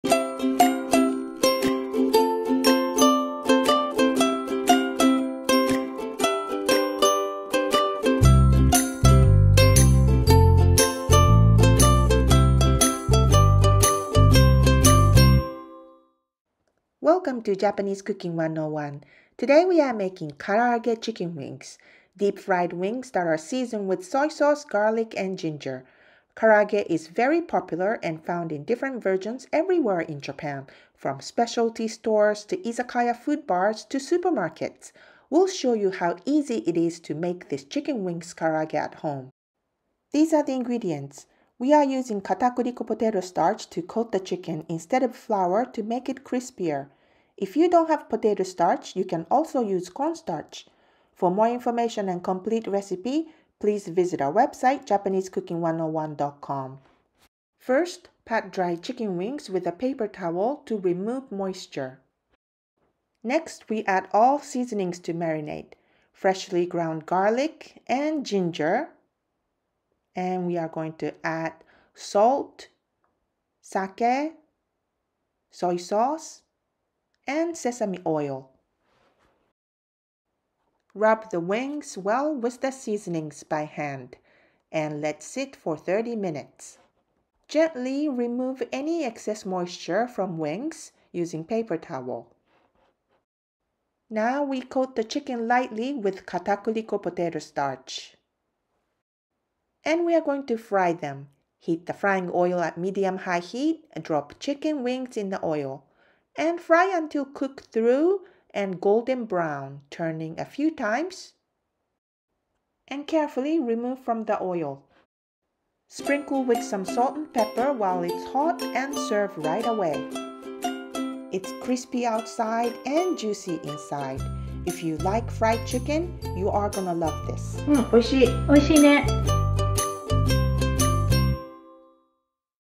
Karaage Chicken Wings Recipe - Japanese Cooking 101
We will show you how to make Karaage Chicken Wings (手羽先の唐揚げ), deep-fried chicken wings that are seasoned with soy sauce, garlic, and ginger. Karaage (Japanese fried chicken) is very popular and  found in different versions everywhere in Japan from speciality stores to Izakaya food bars to supermarkets. We’ll show you how easy it is to make this chicken wings Karaage at home.